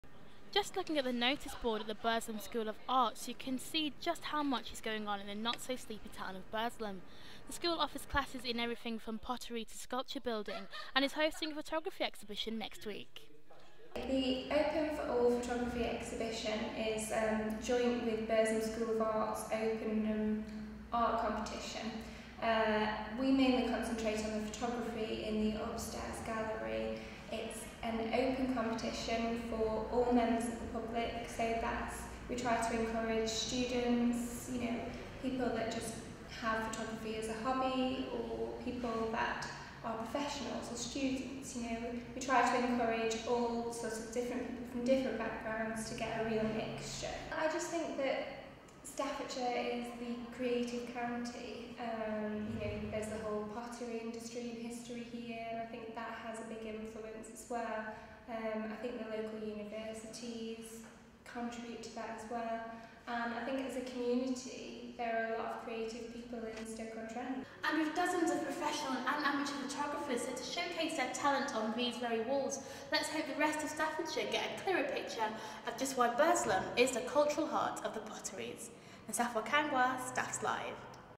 Burslem School of Art Photography Exhibition
Nsofwa Kangwa covers the Photographers Collection's plan to bring a public photography exhibition to the Burslem School of Arts.